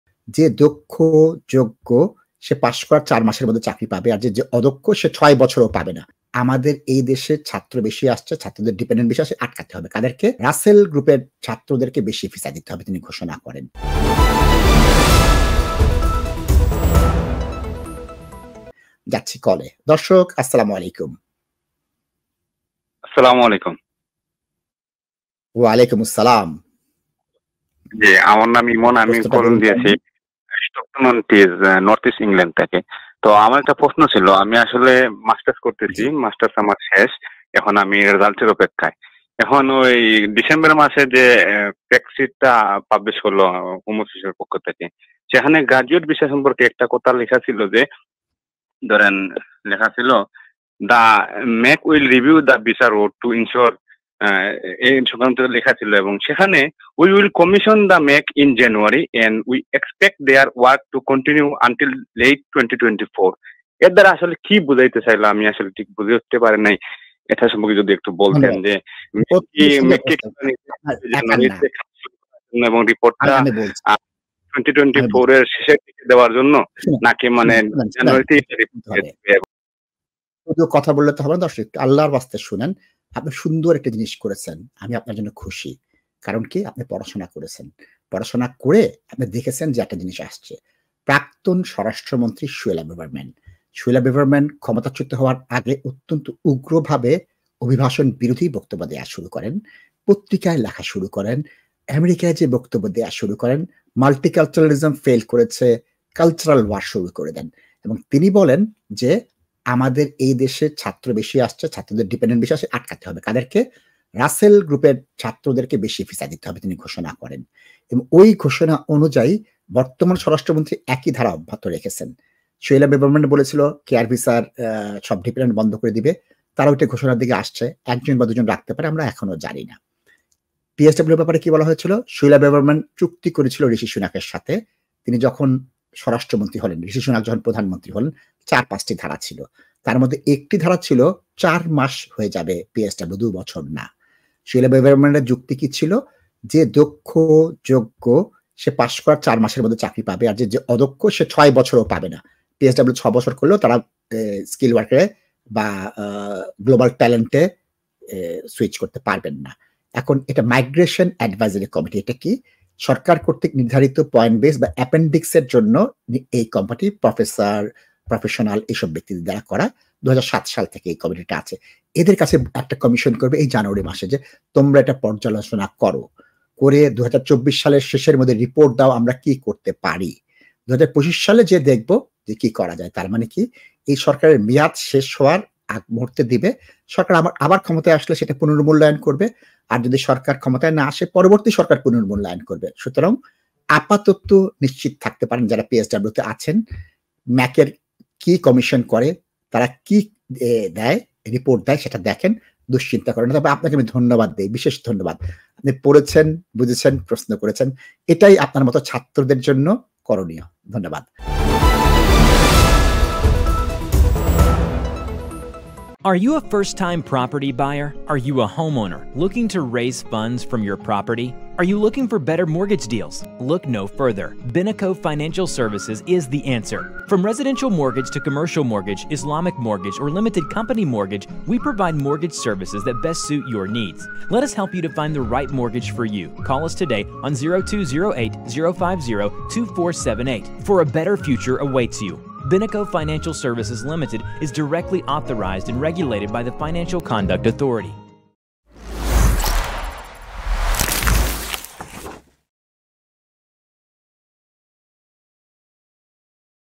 Post Study Work - ভিসার মেয়াদ কি কমিয়ে দিবে?
PSW সাধারণত দুই বছর মেয়াদি ভিসা সিস্টেম হিসেবে চালু ছিল। সরকার এই ভিসা পর্যালোচনা করতে যাচ্ছে। এই ভিডিওতে বিস্তারিত আলোচনা করা হয়েছে।